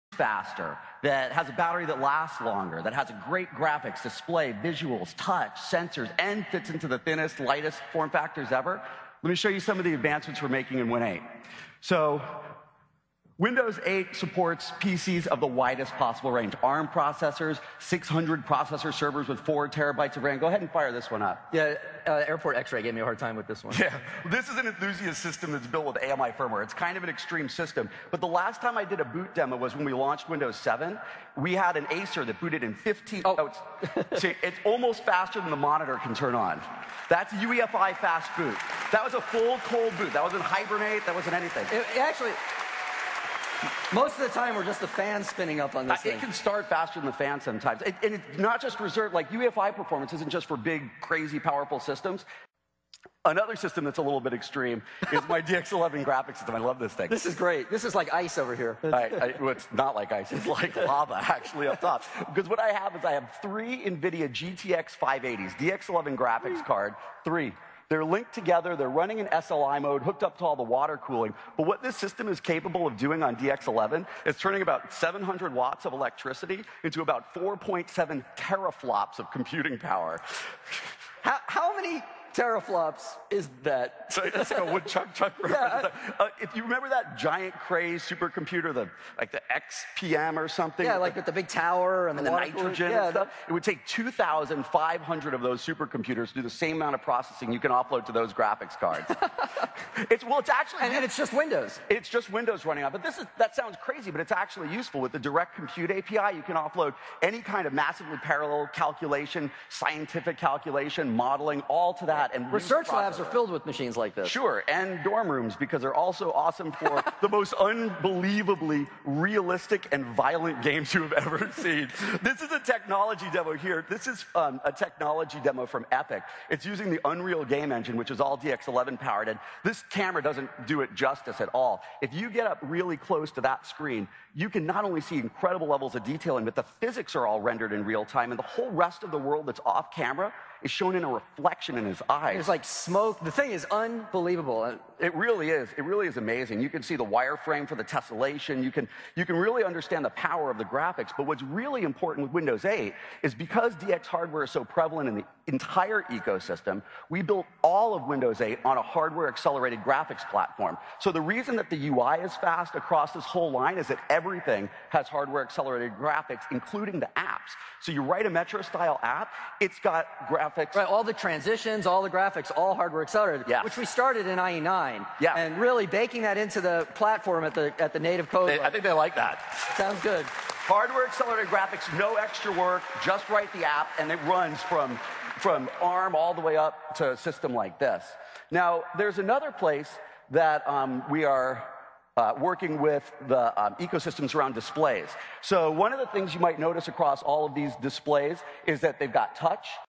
Windows 8 boot up on a high end pc
This is a video from the Micosoft build confrence that shows Windows 8 boot times on a high end pc. I have also added another part of the conference where they show another high end pc and talk a little bit about graphics enjoy :)